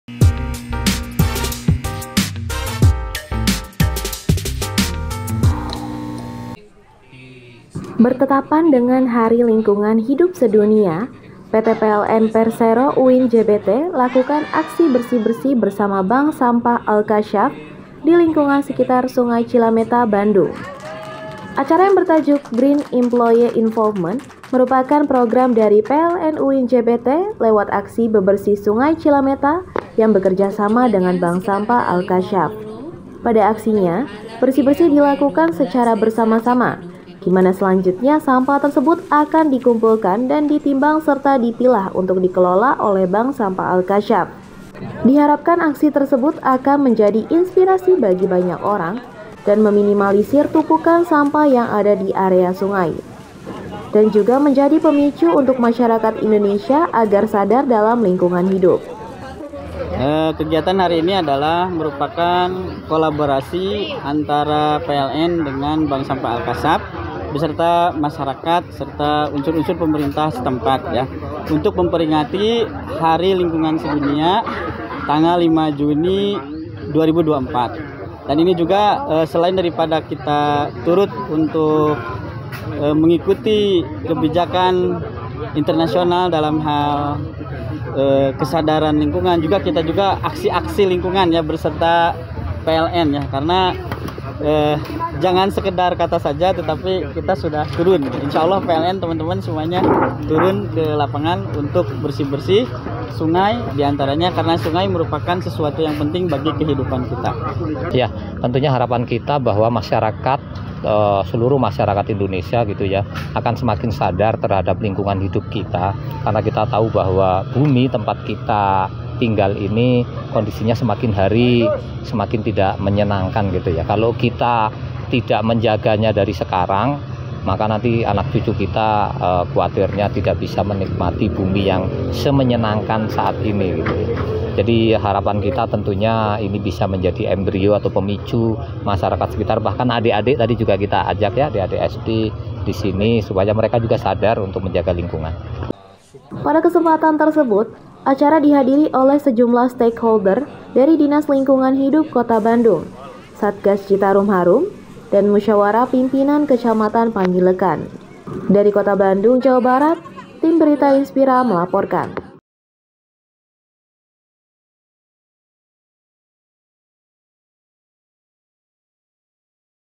PLN UIN JBT BERSAMA BANK SAMPAH AL-KASYAF LAKUKAN AKSI BAKTI SOSIAL
PLN UIN JBT bersama Bank Sampah Al-Kasyaf menggelar aksi bersih-bersih di Sungai Cilameta, Bandung, dalam memperingati Hari Lingkungan Hidup Sedunia pada Rabu, 5 Juni 2024